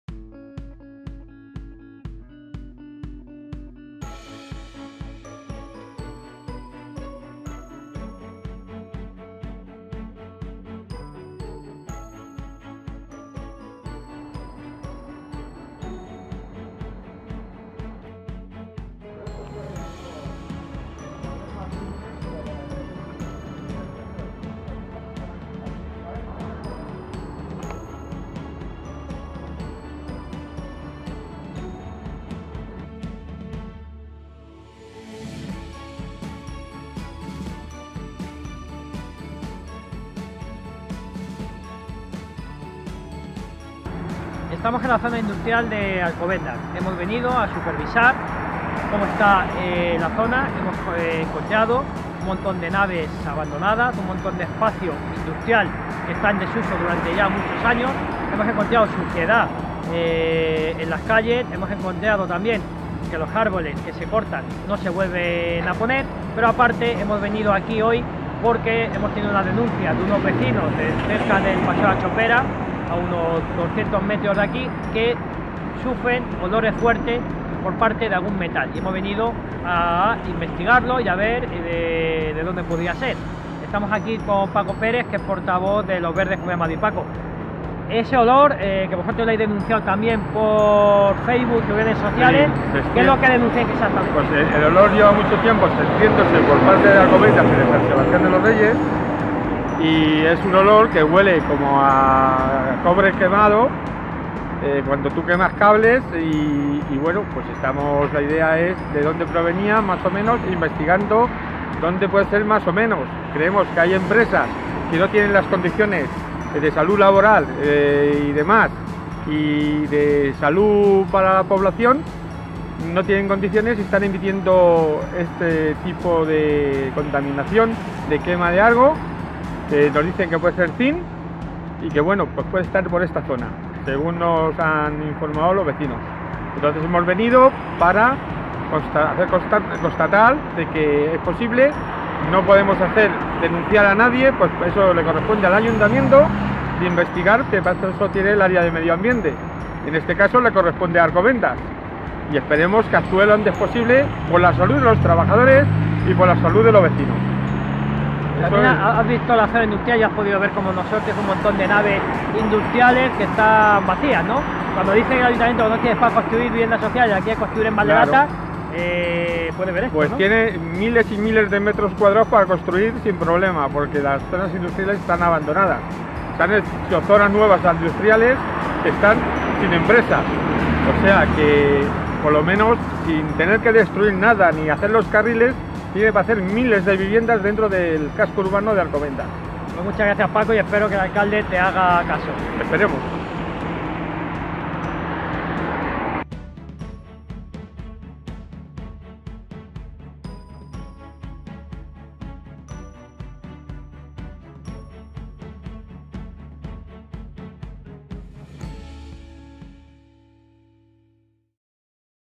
OLORES ZONA INDUSTRIAL ALCOBENDAS
OLORES  ZONA  INDUSTRIAL ALCOBENDAS, Empresas abandonadas, olores, suciedad los vecin@s de Alcobendas se quejan del estado del polígono industrial histórico de la localidad.
SÍ SE PUEDE y Los Verdes han estado visitándolo. Un corto de Uwe Süssmann, SÍ SE PUEDE